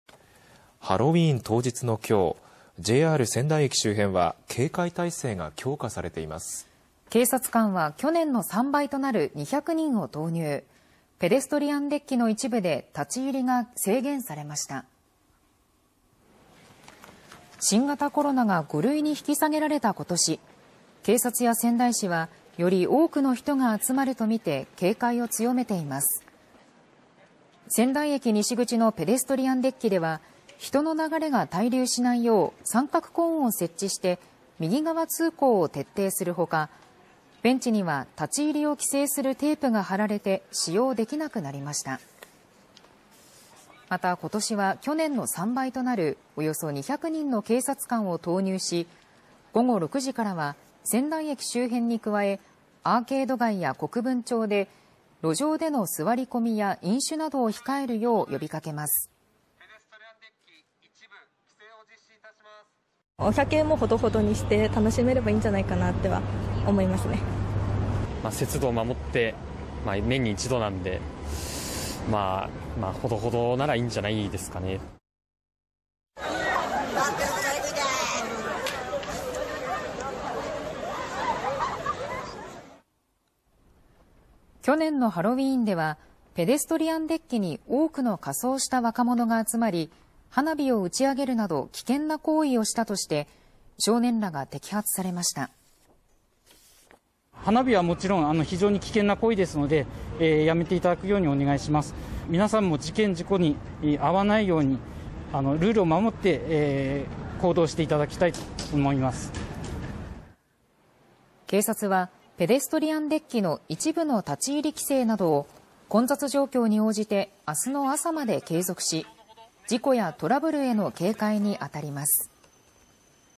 ハロウィーンで仙台駅周辺の警戒態勢を強化 警察官２００人を投入
警察官を前年の３倍となる２００人投入し、ペデストリアンデッキの一部で立ち入りが制限されました。